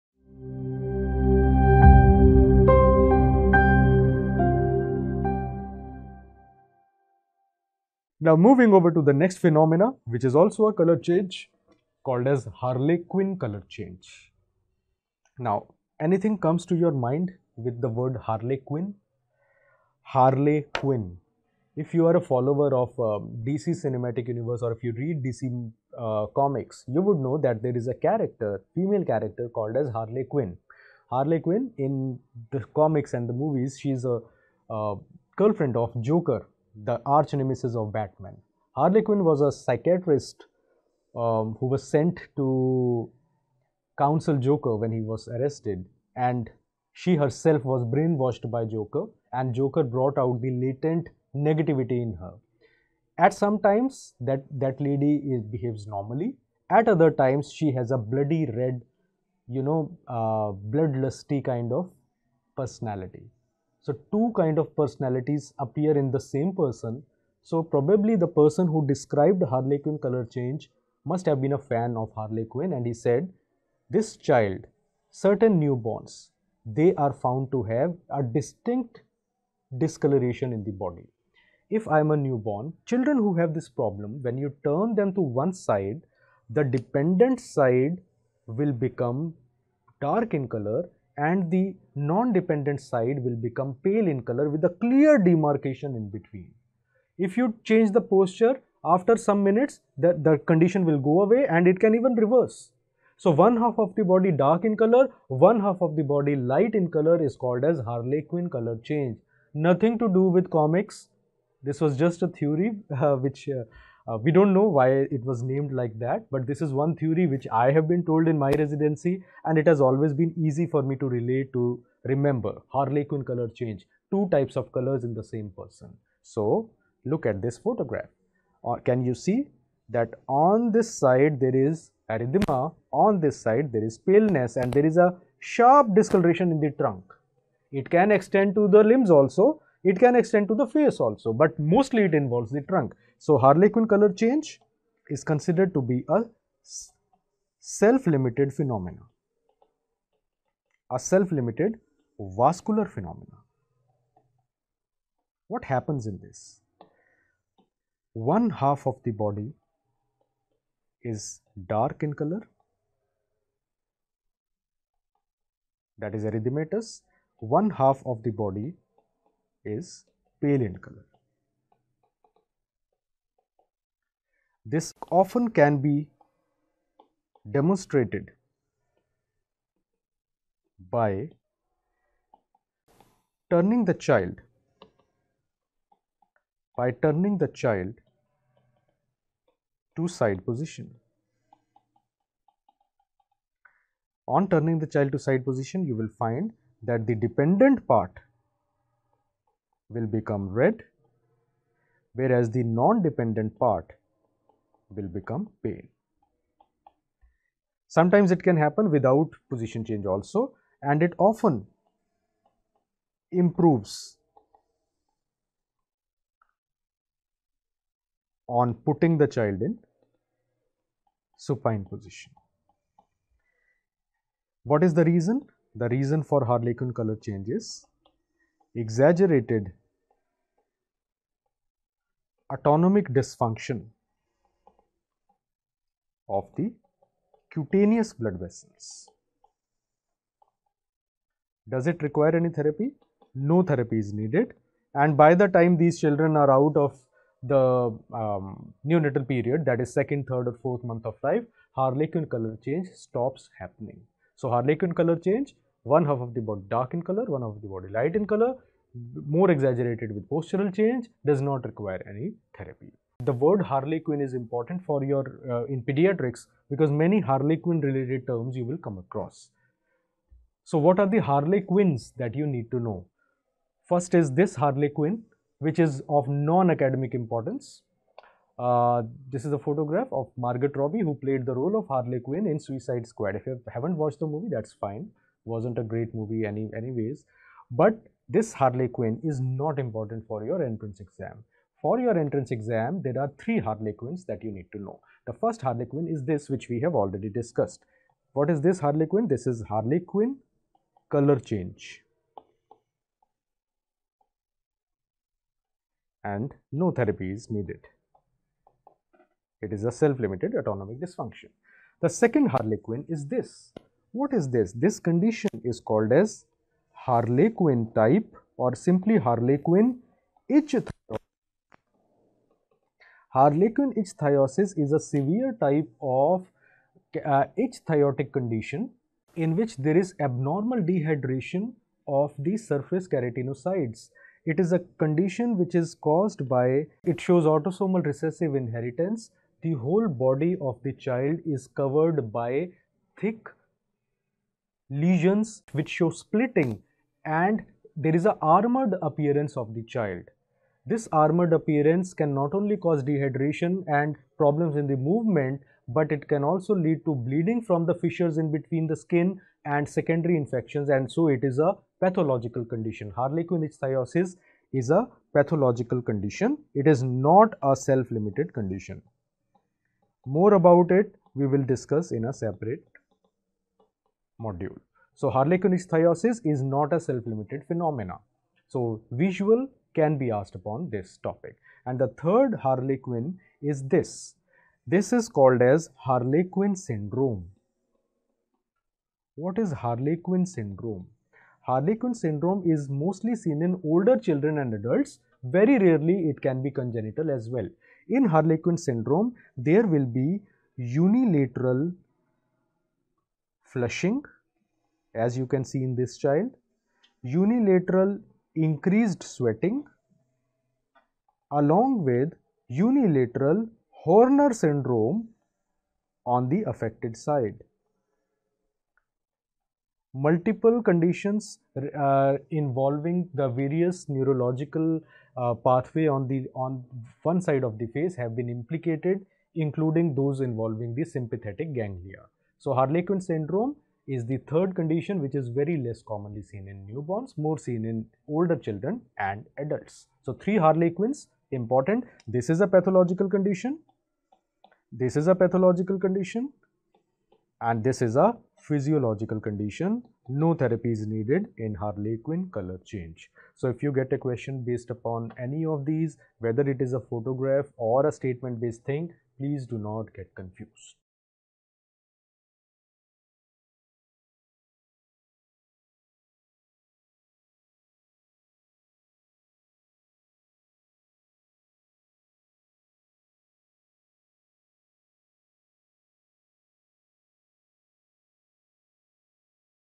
Harlequin Colour Change by Dr. Sandeep Sharma
Watch Dr. Sandeep Sharma's video on Harlequin Colour Change, a valuable resource for NEET Super Specialty in Pediatrics preparation. Gain insights into managing this condition effectively, equipping yourself with the knowledge needed for success in the exam.